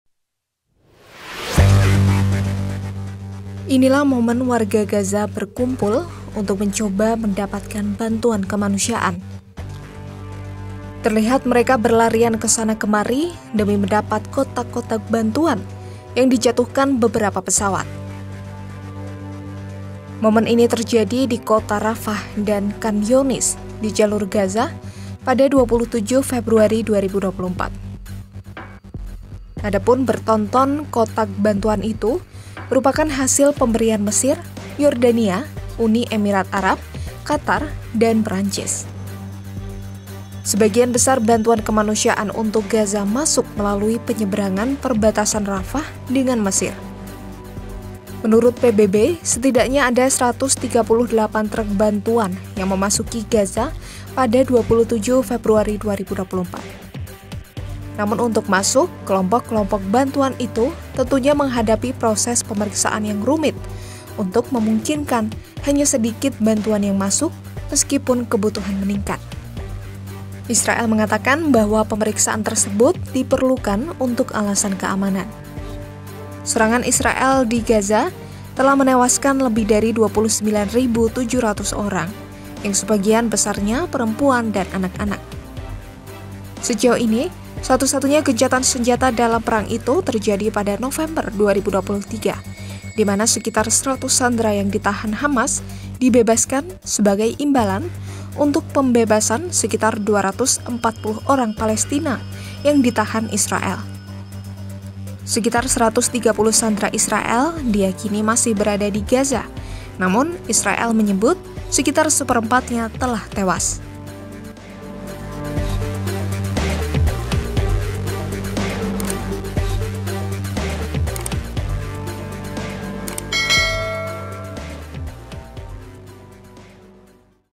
Momen Warga Gaza Berkumpul untuk Tangkap Bantuan dari Helikopter
Sejumlah pesawat menjatuhkan kotak-kotak bantuan ke Kota Rafah dan Khan Younis di Jalur Gaza pada Selasa (27/2/2024). Itu adalah tempat ribuan warga Gaza mengungsi akibat serangan Israel. Mereka pun berkumpul untuk mencoba mendapatkan bantuan yang dijatuhkan dari langit di atas Gaza.

Militer Mesir berkata, Mesir, Yordania, Uni Emirat Arab, Qatar dan Perancis menjatuhkan berton-ton bantuan kemanusiaan di atas Gaza. Sebagian besar bantuan kemanusiaan untuk Gaza dikirim melalui pintu perbatasan Rafah, yang berbatasan dengan Mesir.

PBB mengatakan sebanyak 138 truk bantuan memasuki Jalur Gaza pada Selasa. Lembaga-lembaga bantuan mengaku menghadapi proses pemeriksaan yang rumit yang pada akhirnya hanya memungkinkan sedikit bantuan untuk masuk ke Gaza meski kebutuhan di sana sangat besar. Israel mengatakan pemeriksaan tersebut perlu dilakukan untuk alasan keamanan.

Penulis : Irawan Sapto Adhi
Penulis Naskah: Rizkia Shindy
Narator: Rizkia Shindy
Video Editor: Rizkia Shindy
Produser: Yusuf Reza Permadi